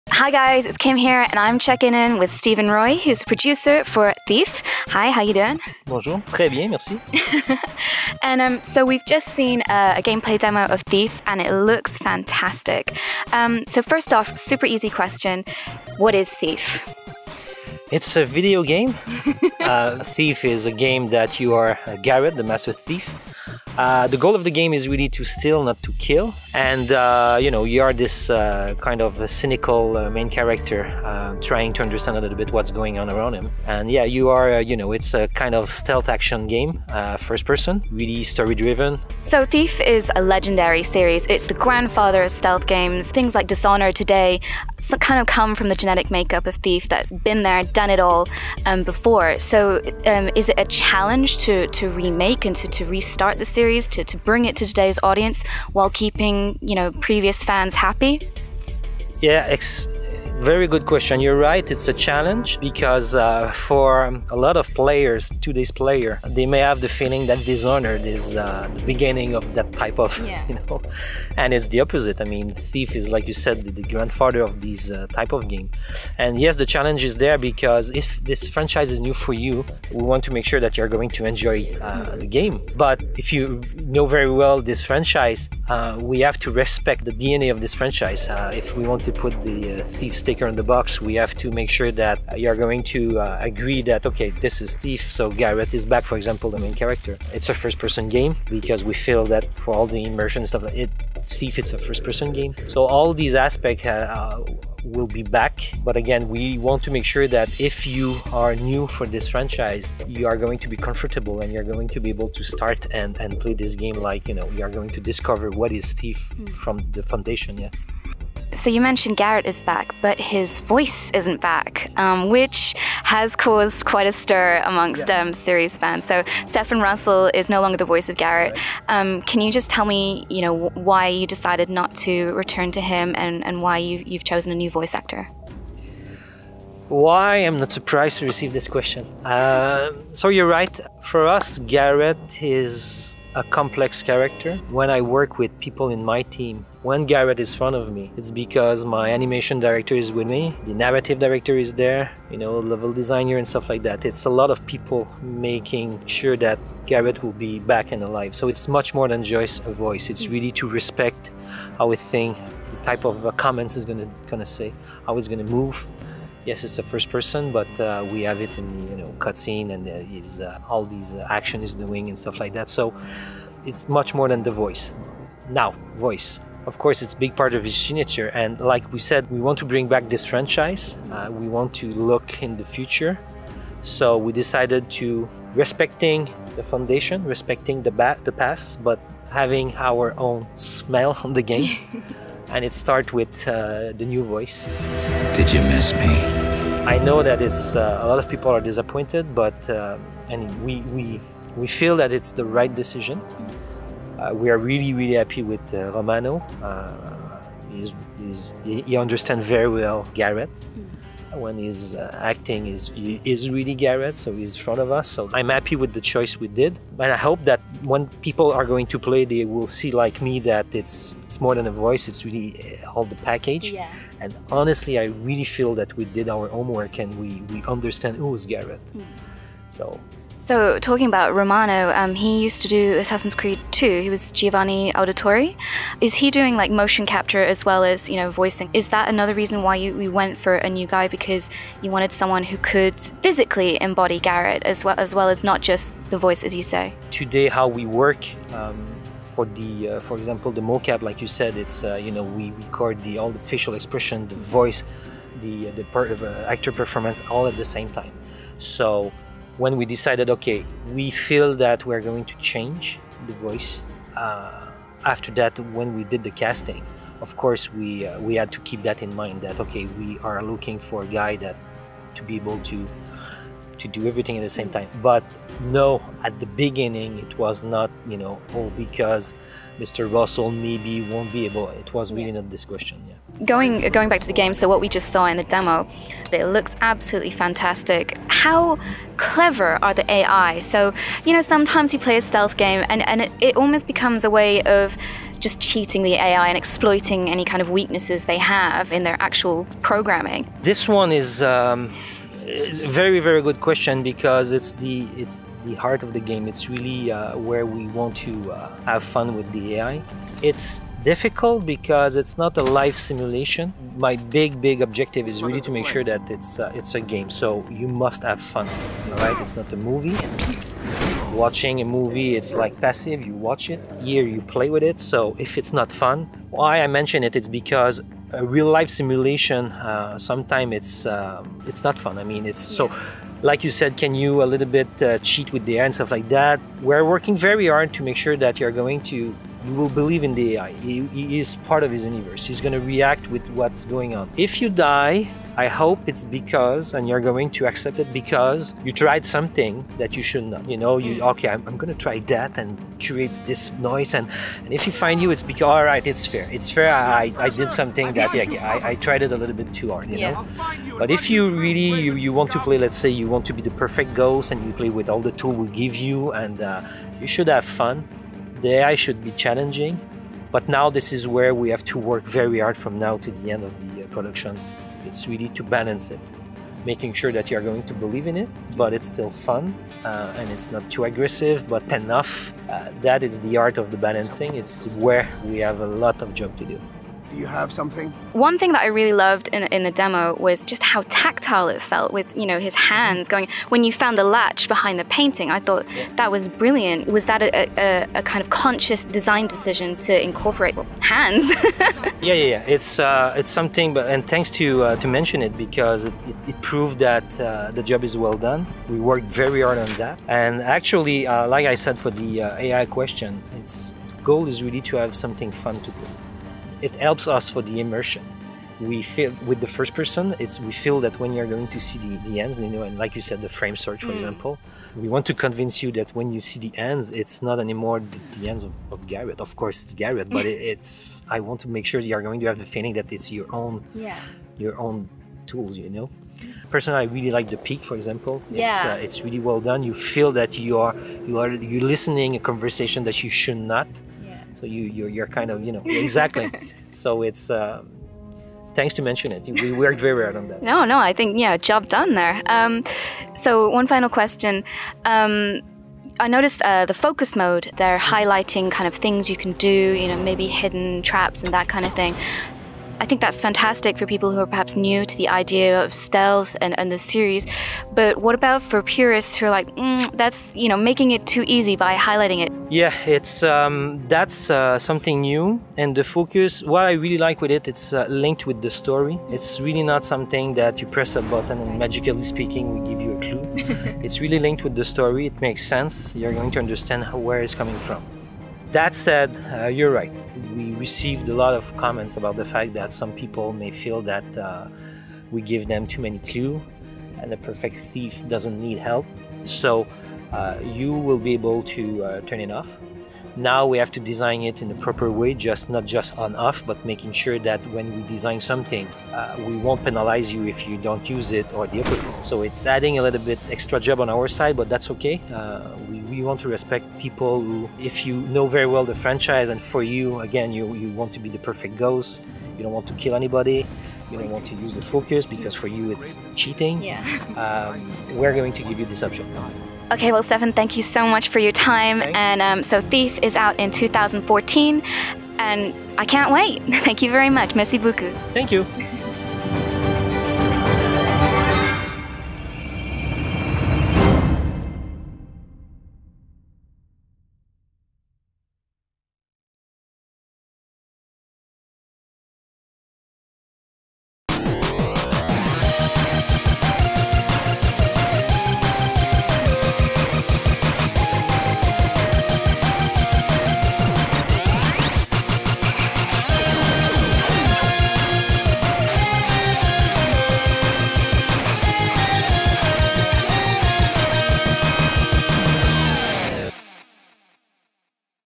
YOGSKIM Special! Thief - INTERVIEW
Kim checks in with Stéphane Roy, Producer for the very exciting Thief! Tune in as we talk about Thief's return, Garret's voice change and much more! Thief is due out in 2013 on PlayStation 4, Xbox One and PC.